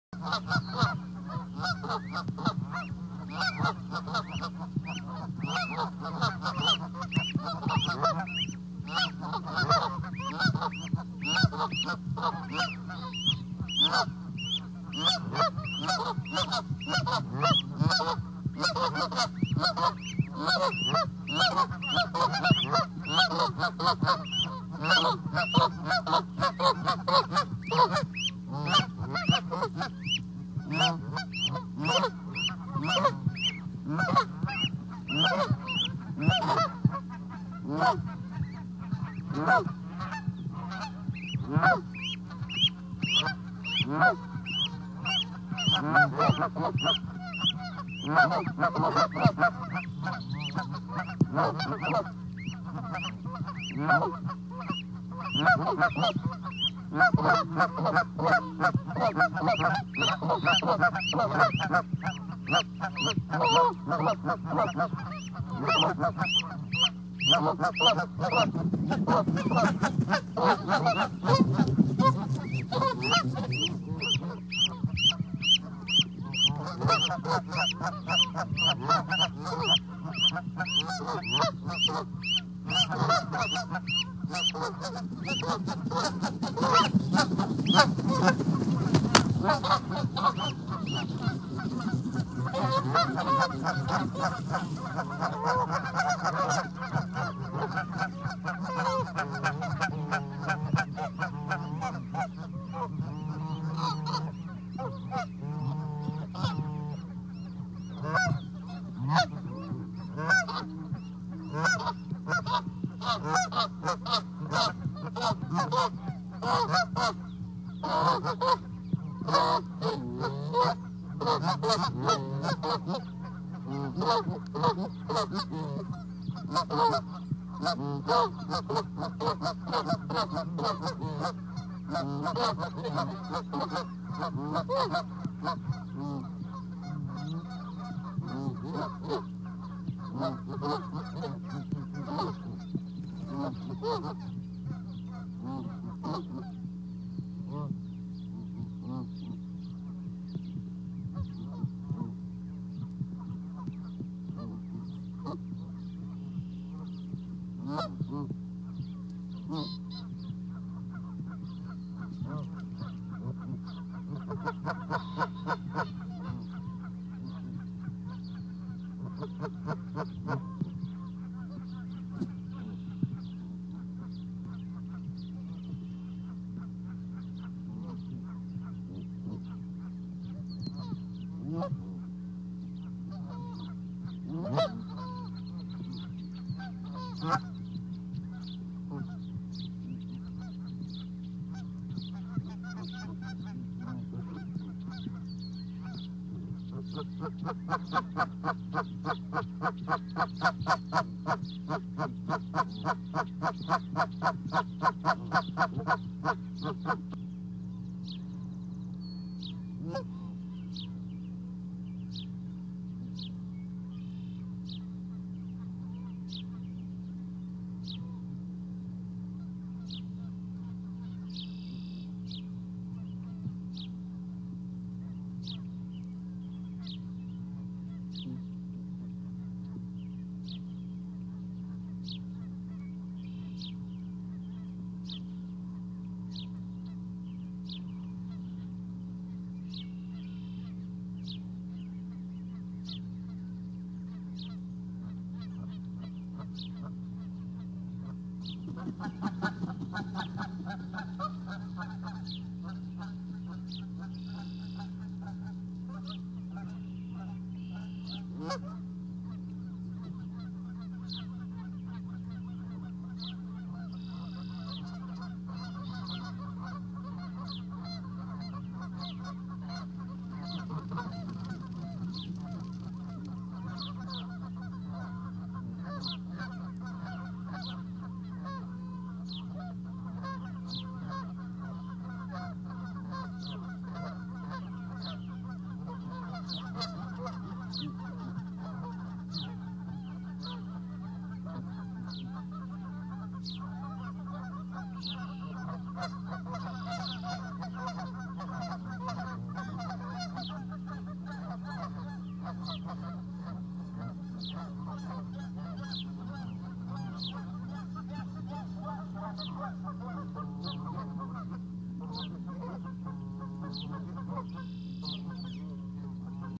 Osprey Nest Charlo Montana Goslings take a leap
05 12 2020
Goslings  take a leap
Amazing to watch Parents call them  and they jump
Courtesy of Owl Research Institute